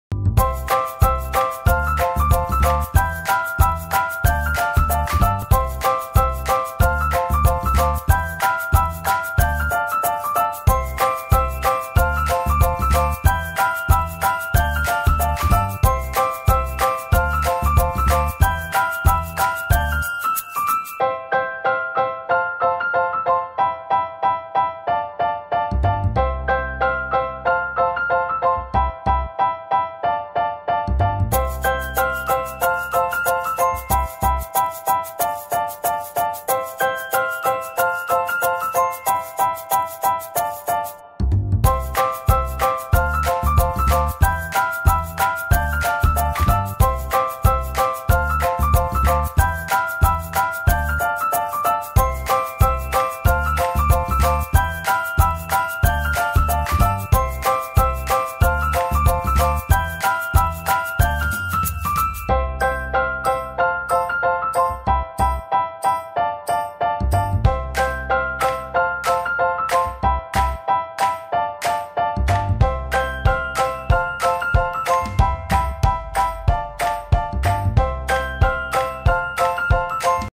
How to simple draw and paint a baby SHEEP for KIDS
Kids love to draw simple drawings and paint them are great start to introduce them to animal planet :) How to simple draw and paint a baby sheep for kids ? Watch out in this video